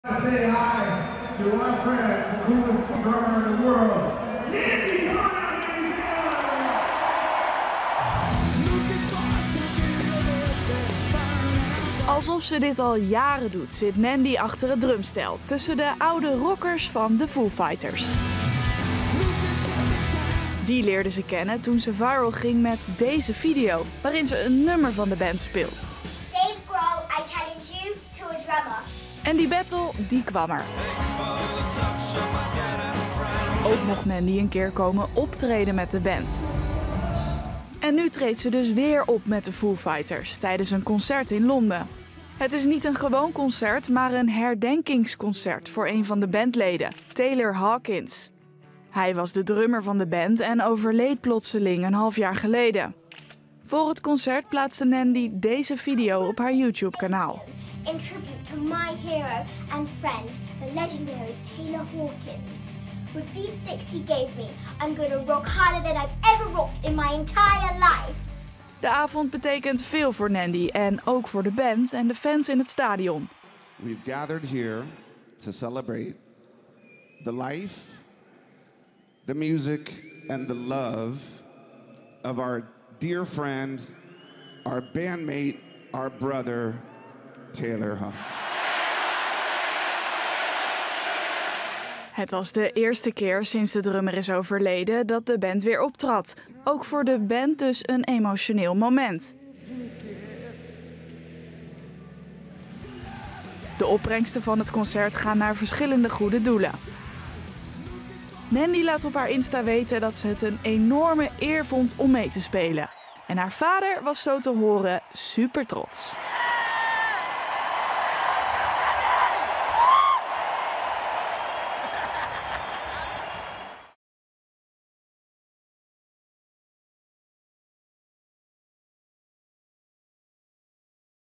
Nandi (12) drumt met beroemde band tijdens herdenkingsconcert
In een uitverkocht stadion mocht Nandi van 12 drummen met een van haar favoriete bands: de wereldberoemde Foo Fighters. Nandi leerde de band kennen toen ze viral ging met een video waarin ze een nummer van de band drumde. Nu mocht ze voor de tweede keer samen met de band een show geven. Het was een herdenkingsconcert voor een van de bandleden: Taylor Hawkins. Hij was drummer van de band en overleed een half jaar geleden plotseling.